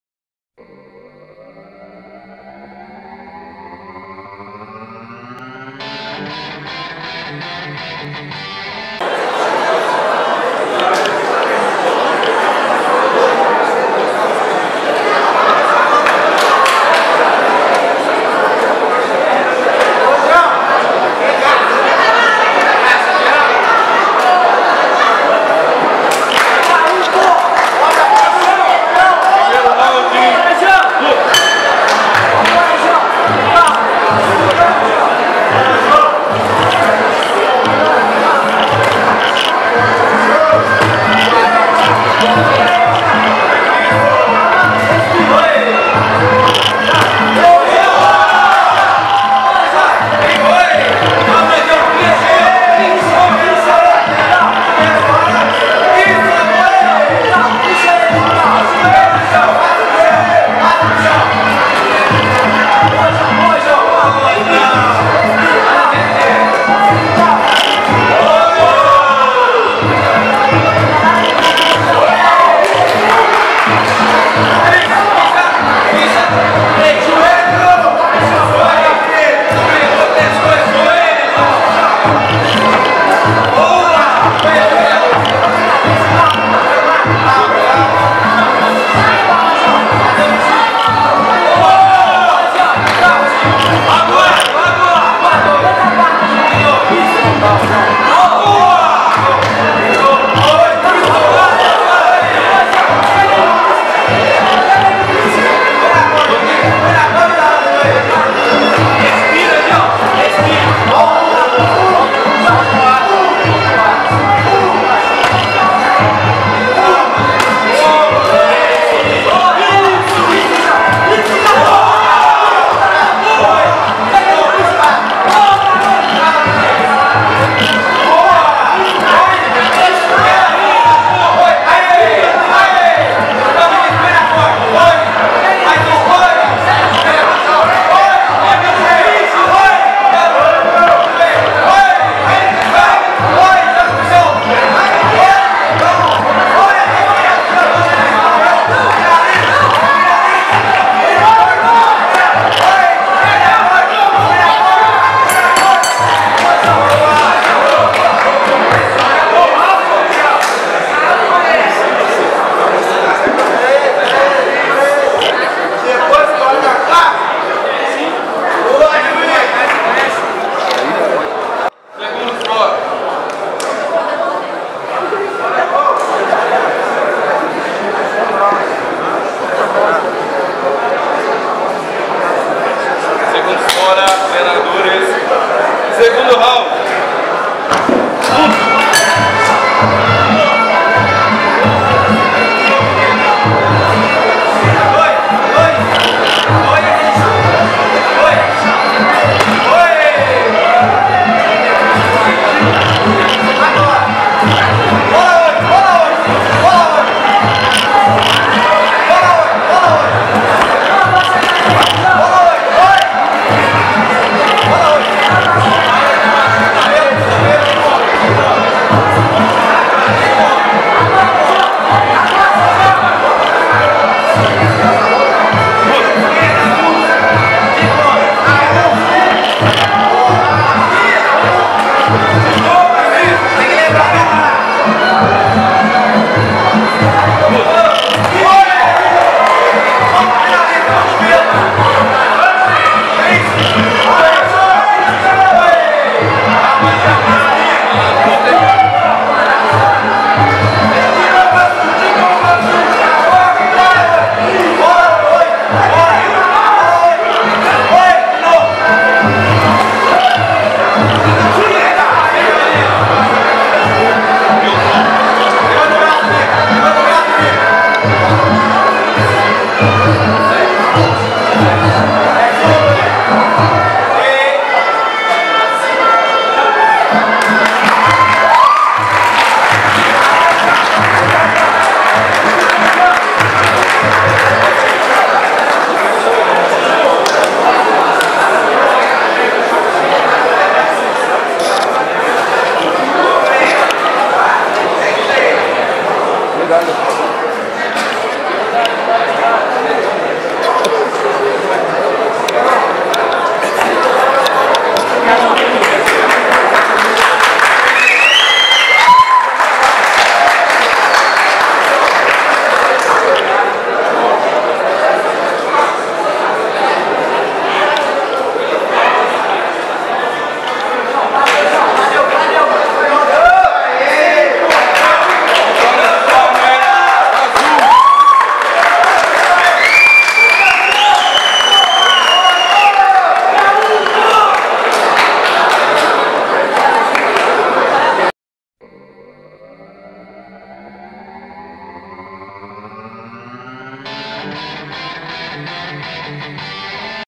Vinicius vs João - Contagem Fight Night 6
Em combate de MuayThai amador até 55kg, Vinicius (Tiger Force Team) venceu João (Projeto K1) por nocaute técnico no segundo round.